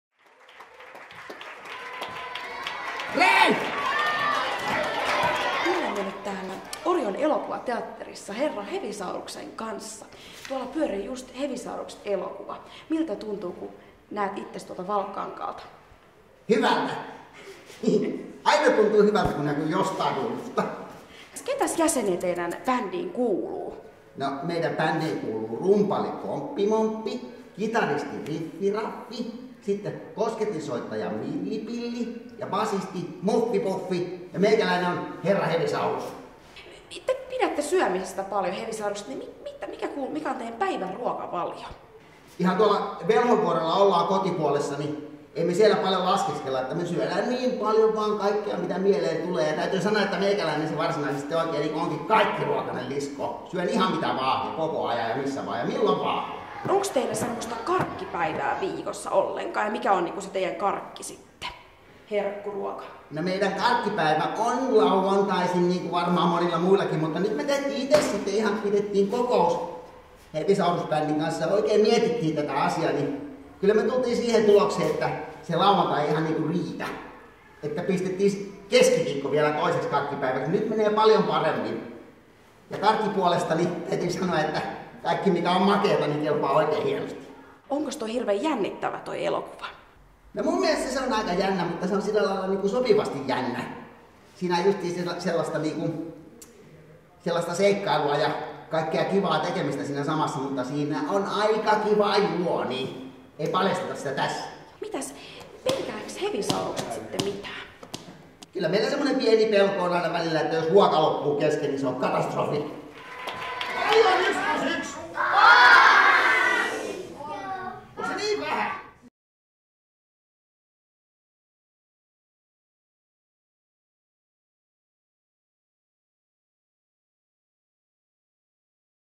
Rakkautta & Anarkiaa 2016: Kaikkiruokainen Hevisaurus pelkää vain yhtä asiaa
Itse Herra Hevisaurus vieraili Hevisaurus-elokuvan R&A Pulpettikino -näytöksessä Elokuvateatteri Orionissa. Syömistä rakastava muinaisolento oli tyytyväinen elokuvan kuvauksiin, etenkin kun siellä sai syödä niin paljon kuin halusi. Rakkautta & Anarkiaa -festivaaleilla esitettävä Pekka Karjalaisen ohjaama Hevisaurus-elokuva kertoo muinaisten dinosaurusten seikkailusta jännittävässä ympäristössä.

Toimitus: Julia Falck
Kuvaus: Kaisa Paavilainen
Editointi: Julia Falck